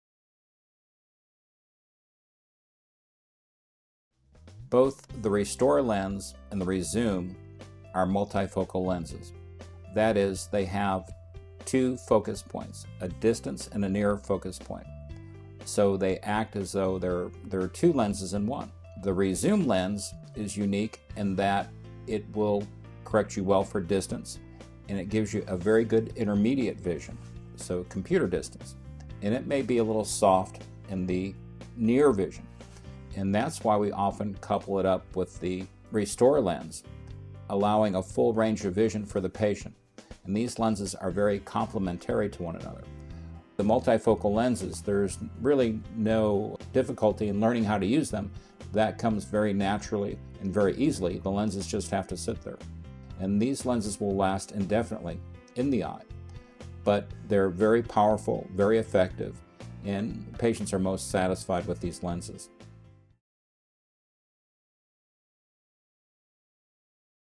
ReZoom™ Lens - Tylock Eye Care and Laser Center serving Dallas and Fort Worth
Our Irving office, Tylock Eye Care and Laser Center, serves our patients in the Dallas / Fort Worth, Texas area with the ReZoom™ lens. The ReZoom™ Lens is made of a series of circular ring areas that are dedicated to focusing either at far or at near. This multi-focal lens is ideal for far & mid distance vision. For more information about ReZoom™ Lens, please

You may contact Dr. Gary Tylock at:

Tylock Eye Care & Laser Center
3100 N. MacArthur Blvd.
Irving, Texas 75062